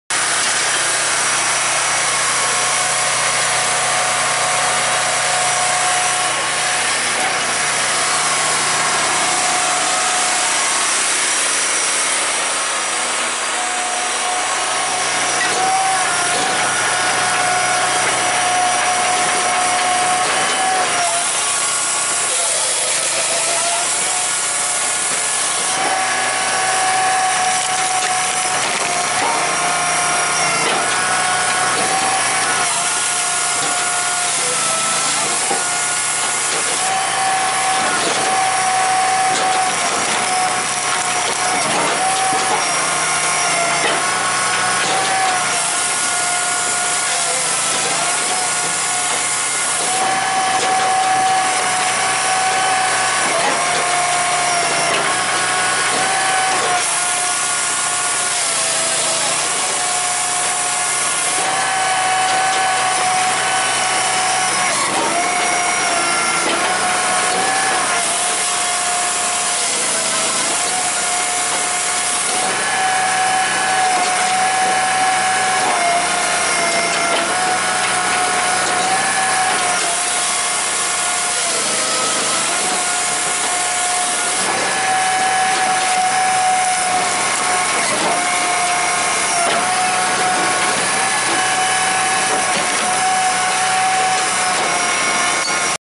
Brute Force 16-24 Firewood Processor with Log Lift
Loading wood processor with log lift & processing log into cut & split firewood.  The log lift converts to live deck and is standard equipment on this model processor.  **This video shows our old style machine with the manually adjustable wedge & slower gear motor for the saw.  Our new model 16-24 comes standard with a hydraulically adjustable splitting wedge & much faster piston saw motor.